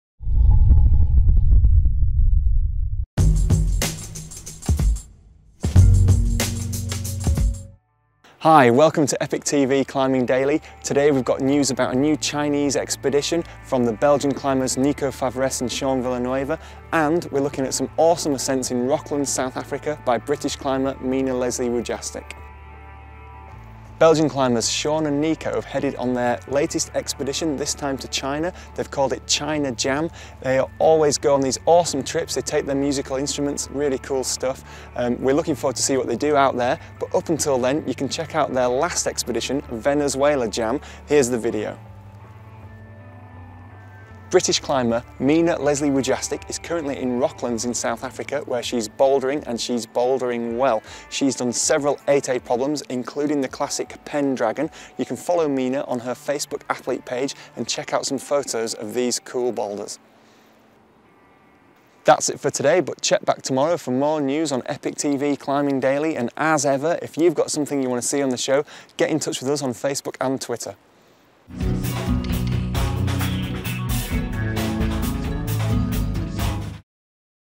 Nicolas Favresse and Sean Villanueva Go Big in China | EpicTV Climbing Daily, Ep. 115
Rock Climbers Nicolas Favresse, Sean Villanueva O'Driscoll, Stéphane Hanssens and Jean-Louis Wertz are heading out to China for their latest expedition. They are producing a rock climbing video when they are out there.

Check out the previous trailer for their video Jungle Jam, climbing in Venezuela:

British climber Mina Leslie-Wujastyk is currently climbing in Rocklands in South Africa, where she has been bouldering. She has bouldered several 8a problems including the classic Pendragon.